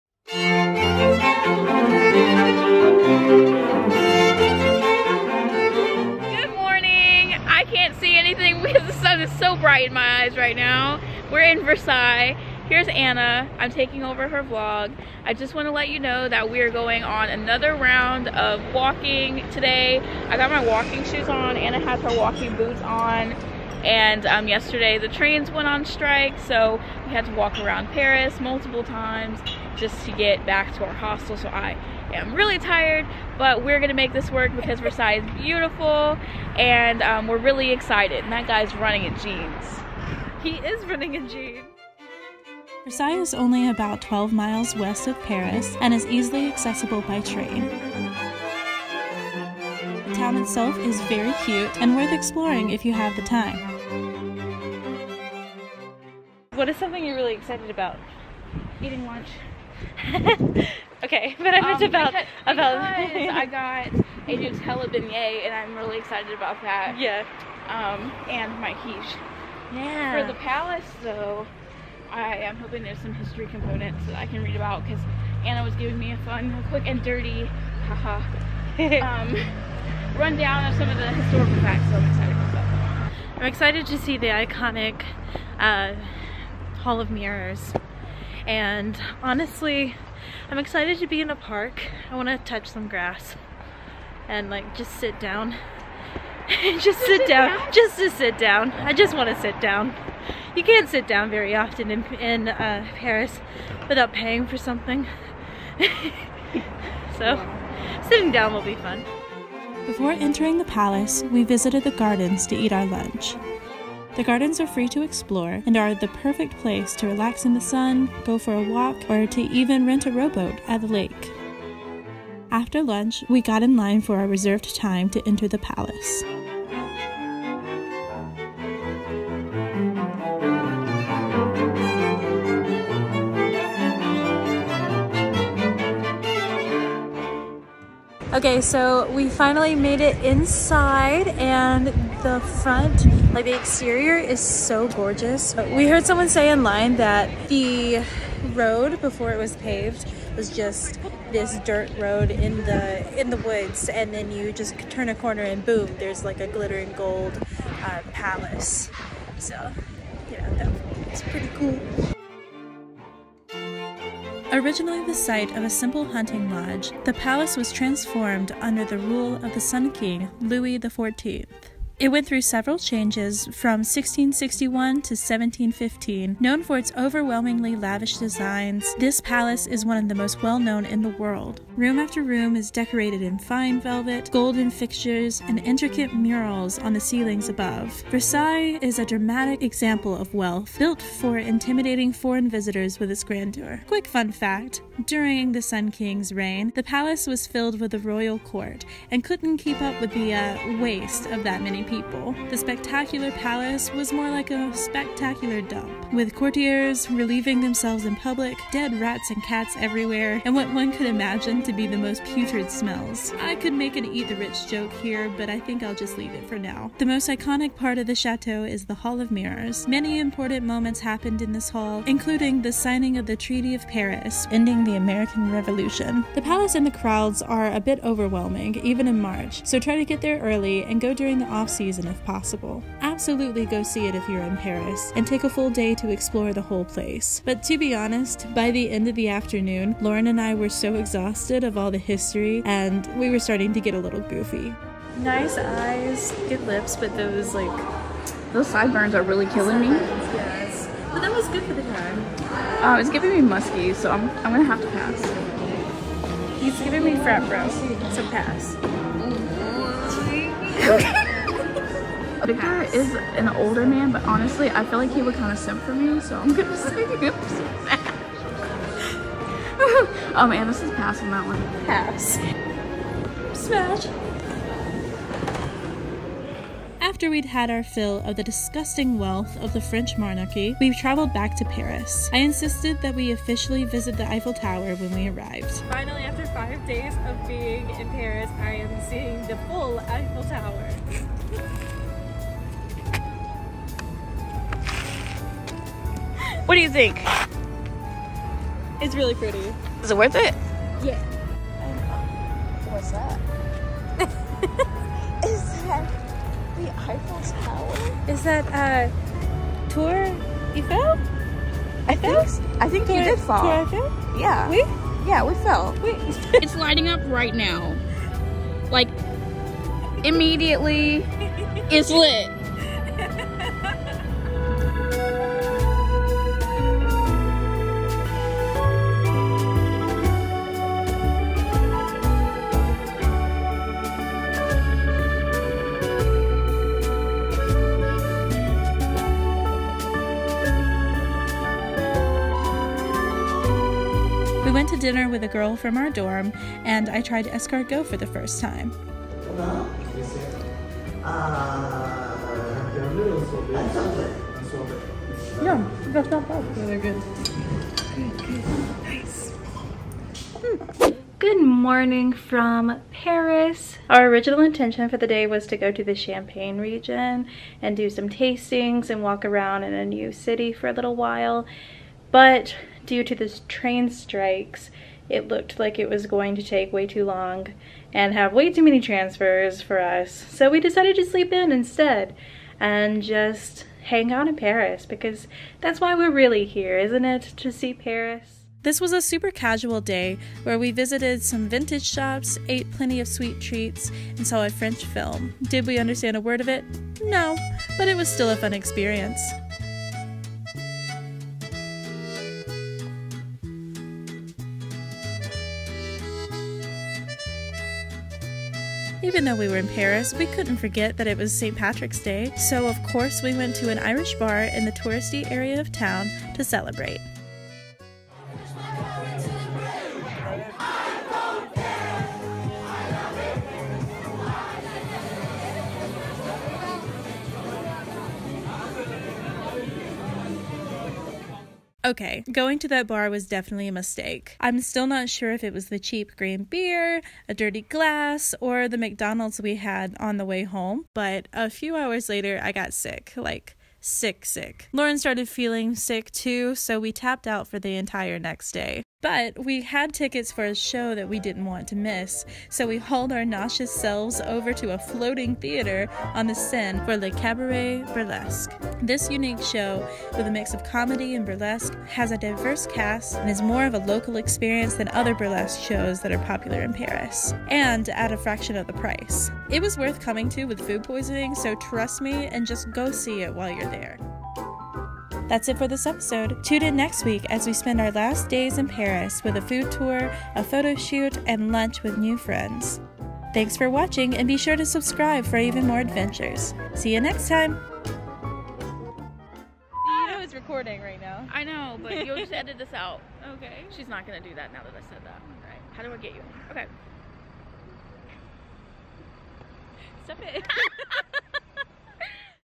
Visiting VERSAILLES 🇫🇷 | Episode 9
In this episode, we take a day trip to the famous Palace of Versailles, basking in all its elaborate glory. We then go visit the Eiffel tower, celebrate Saint Patrick's Day in Paris (???), and go see a burlesque show.

Follow me for live updates on my travels!